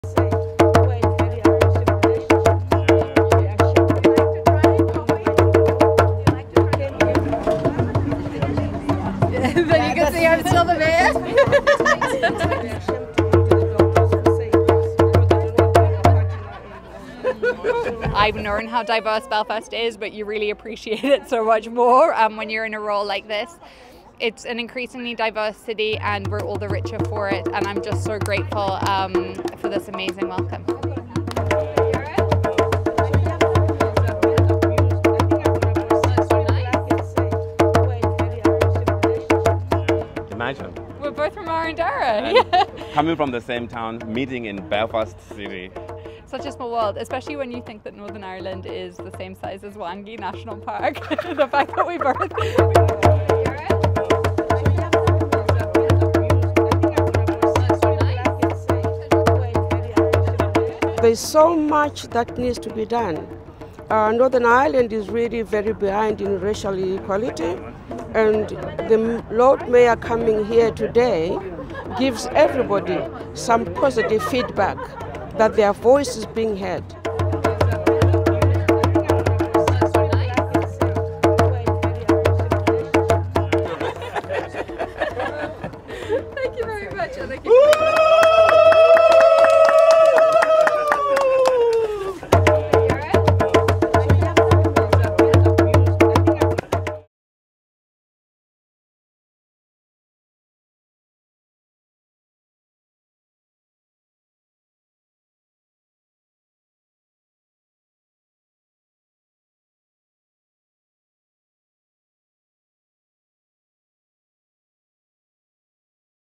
Belfast Lord Mayor Kate Nicholl meets city's African community
Northern Ireland's African community has held a special event to celebrate the installation of the Zimbabwean-born lord mayor of Belfast.
Councillor Kate Nicholl said it was important to show members of all ethnic minorities that they too could one day become lord mayor.